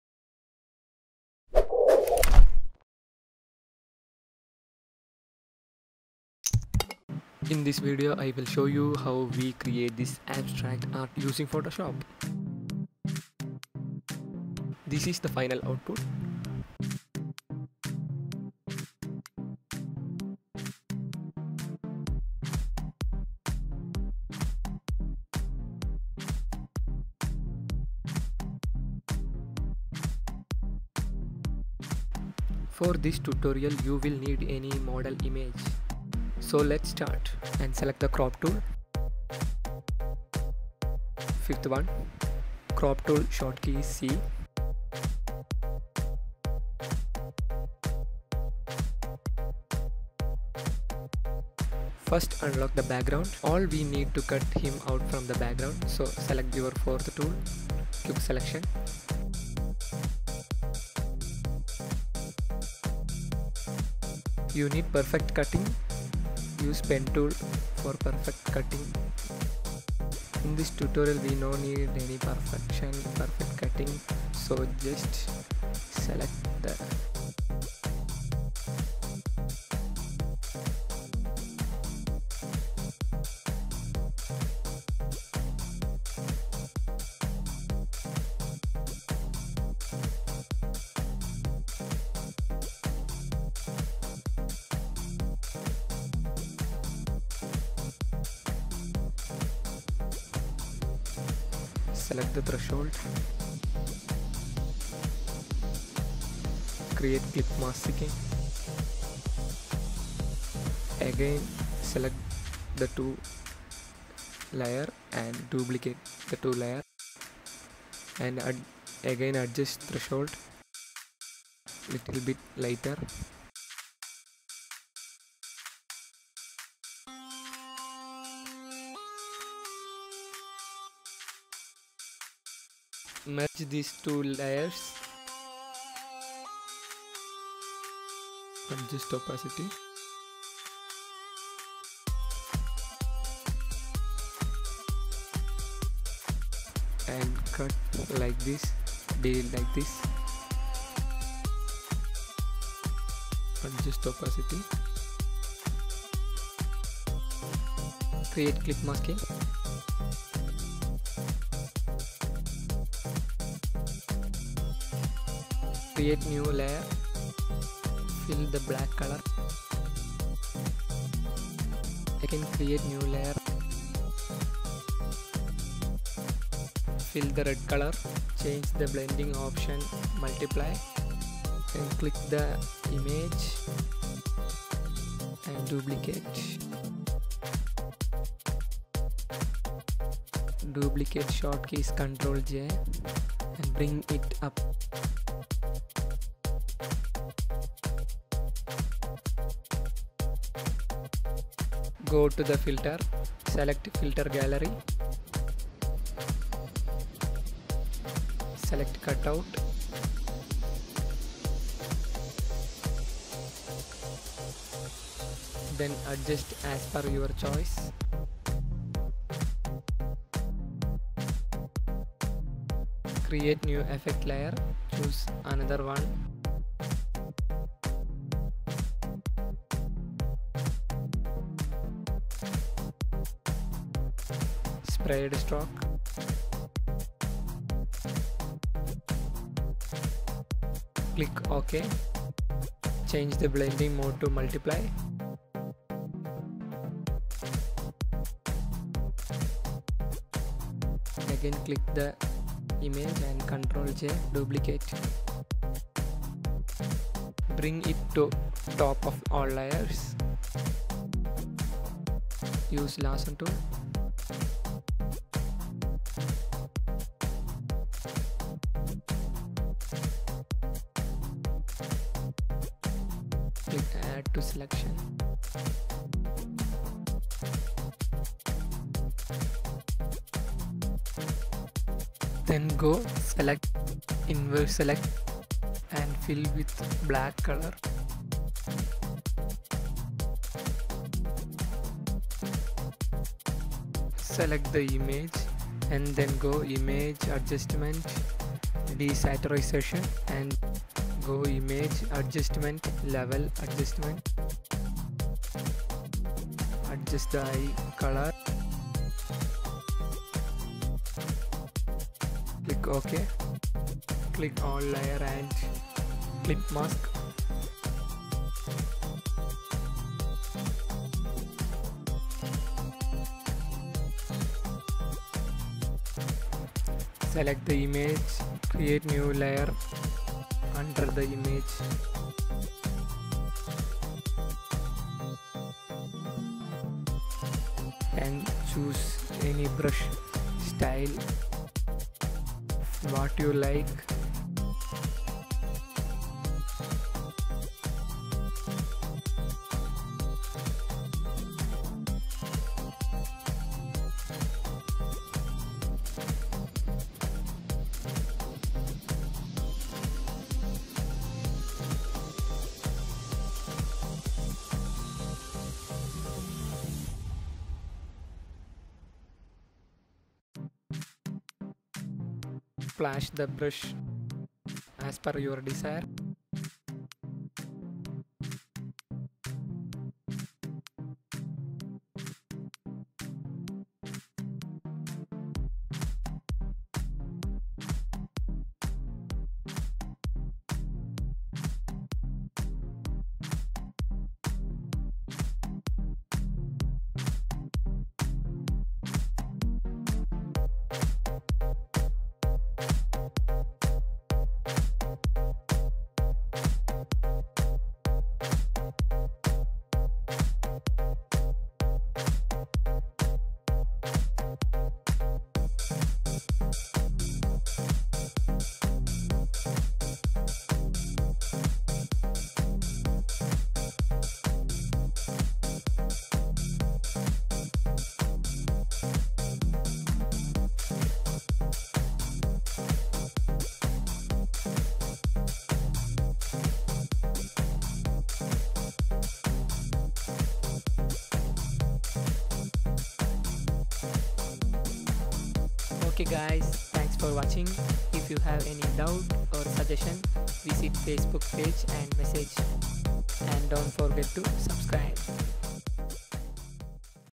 Photoshop Tutorial : Splash Photo Poster Effect | Ink Splatter Blood Kaala Movie Design
Hi, In this video i will show you
how we create this abstract art using photoshop.

I mixed Filter Effect and created some photo effect like this... its super easy, super fun and every time you can try and create different output with same method...

just use different brushes if you do this on different, different layer. i have also shown quickly how you can change the background and we will also use threshold effect like we did cartoon effect tutorial.

its little bit similar to the photo effect that i have done in past but also little different and easy. the photoshop brushes i have used are really good and high resolution but i would recommend trying to find you own set of photoshop brush and experiment with different brushes.

if you have any doubt about installing & Downloading brushes

i am reading your suggestions and ideas and keep coming them.
post or send your creative ideas to us